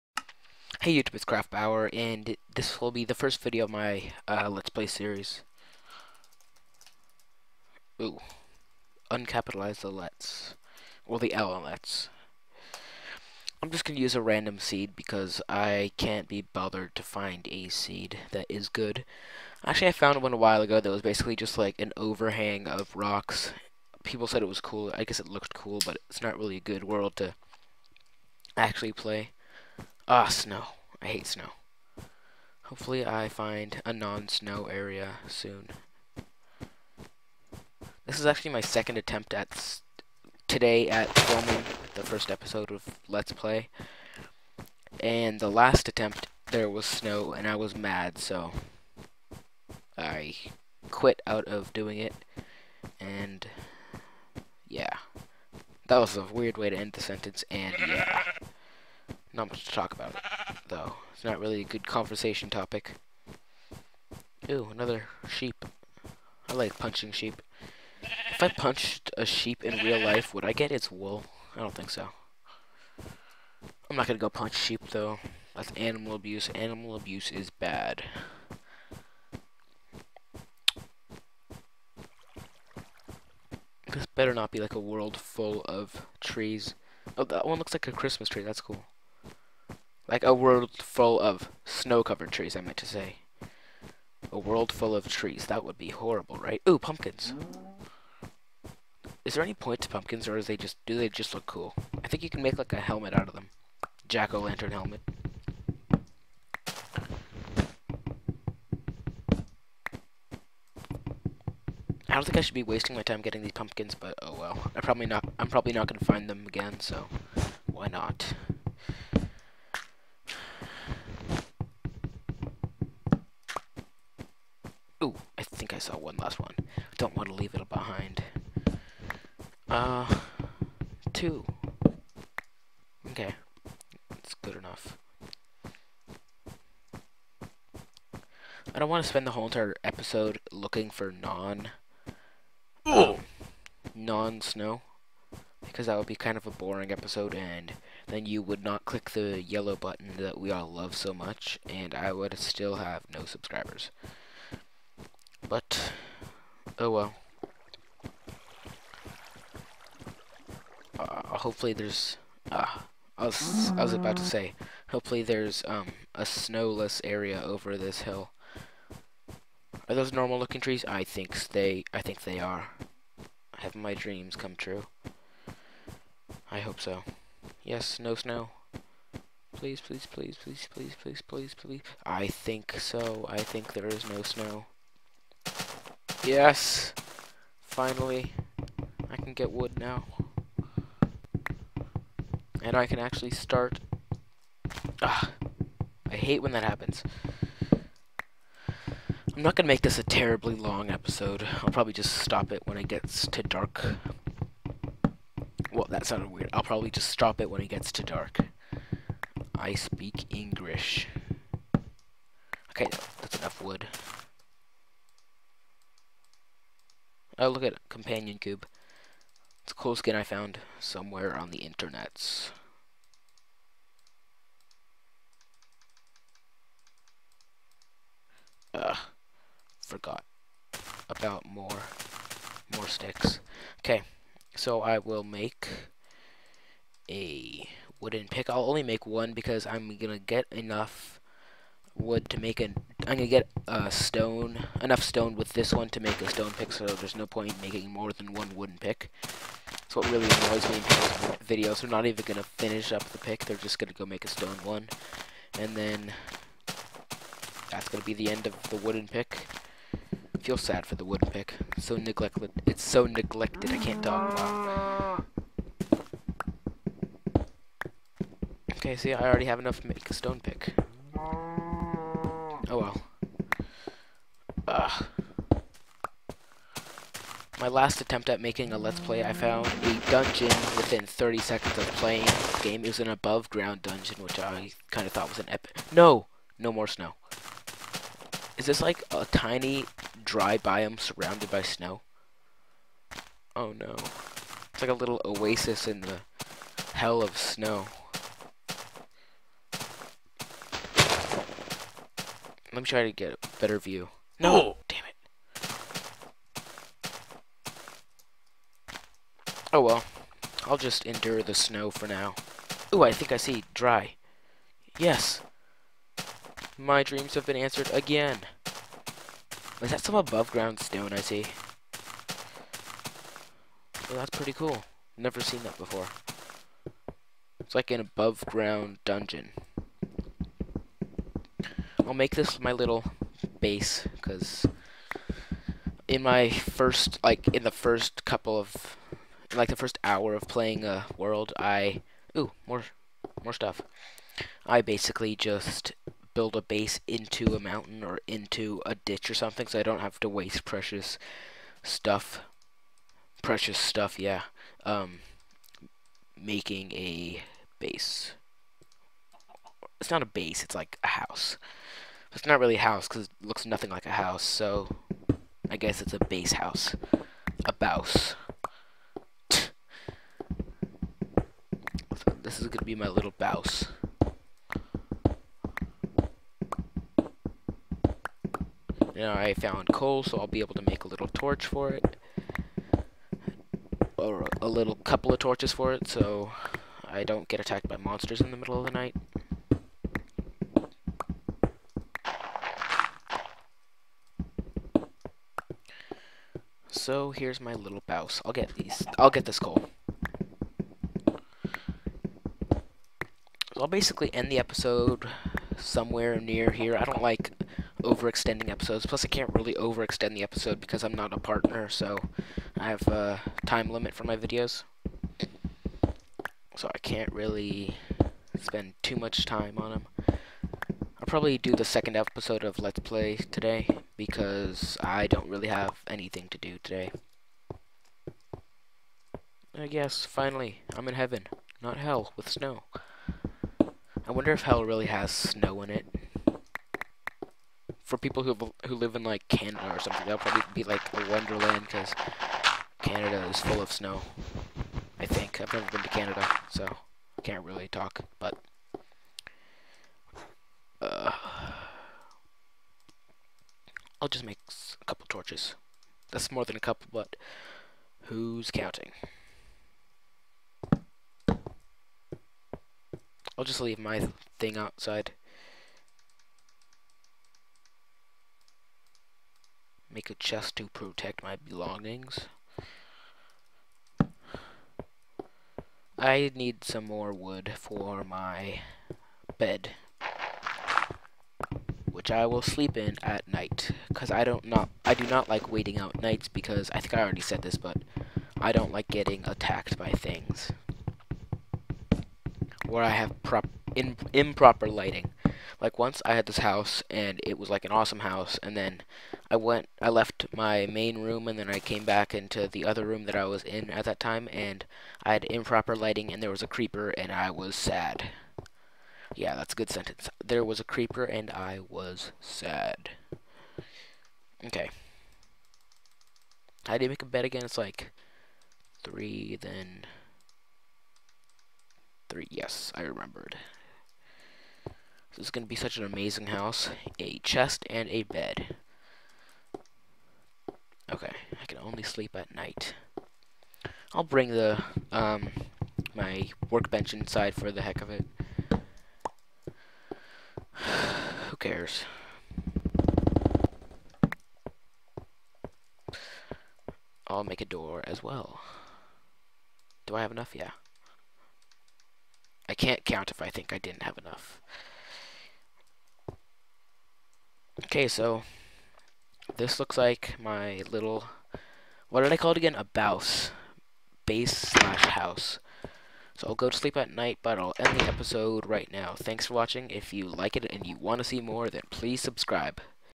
[LET'S PLAY #1] Snow From Hell
Well, this is my first Lets Play. Like any first lets play, it's not terribly exciting. Basically starting out in the world and looking for stuff. The problem is, I started in a world of snow. SNOW FROM HELL!

So, I look around and I finally come across an oasis of non-snow. In that oasis, I build my bouse.

A bouse is my new word for a base/house.

BOUSE.